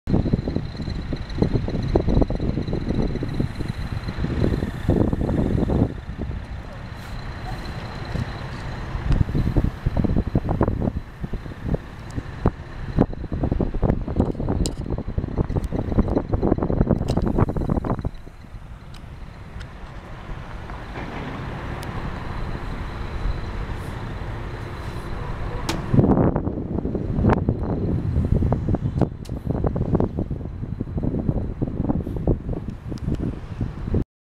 Ponte del mare Pescara
Video dal bellissimo ponte del mare di Pescara. Si vede il porto canale, le montagne ed il mare.